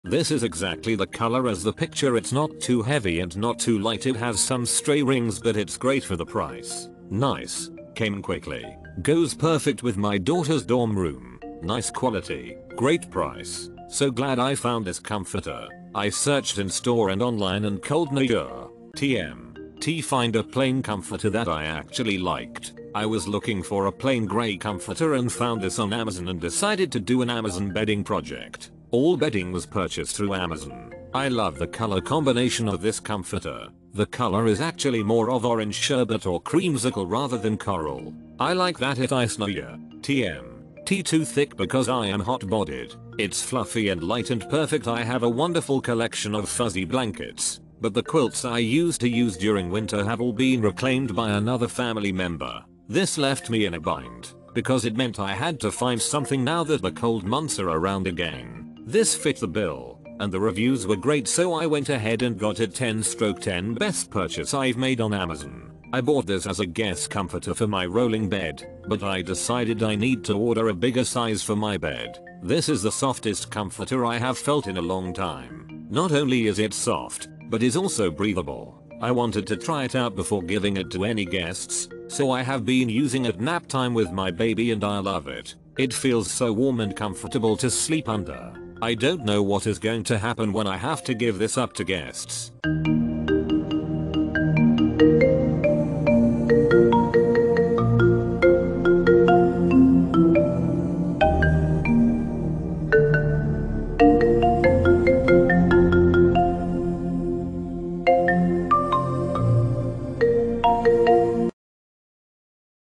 Comfort Spaces Vixie 2 Piece Comforter Set All Season Reversible Goose Down review
Main Features:
100% Microfiber. Imported. PRODUCT FEATURES – Aqua and Grey comforter set, features stitched geometrical pattern. Vacuum sealed for transit protection, space saving and gifting ready while staying lightweight. Comforters are thicker than blankets, while has more fill than quilts. PACKAGE INCLUDES – 1 Soft Down Alternative Comforter, 1 Sham. No need for a duvet cover, but cover can be applied for easy cleaning. MEASUREMENTS – Comforter covers – 66 (W) x 90 (L) inches, 1 Sham cover – 20(W)x26(L) inches each. Measure the mattress to ensure the beds are covered to your satisfaction because standard bed sizes may vary slightly. Use proper sized pillow to ensure it s. EASY CARE - To fluff: Tumble dry on low heat for 15-20 minutes. To Clean: Machine wash cold on gentle cycle, tumble dry on low heat, do not bleach bedset to upkeep quality. Remove from washer and dryer immediately to reduce wrinkles and to fluff. SATISFACTION - We offer 100% satisfaction on our designer bedroom bedding sets with a 30-day free return for our cotton alternative comforters. Available in Aqua / Grey, Black / Grey , Coral / Gray, Navy / Charcoal, Pink / Orange, Pur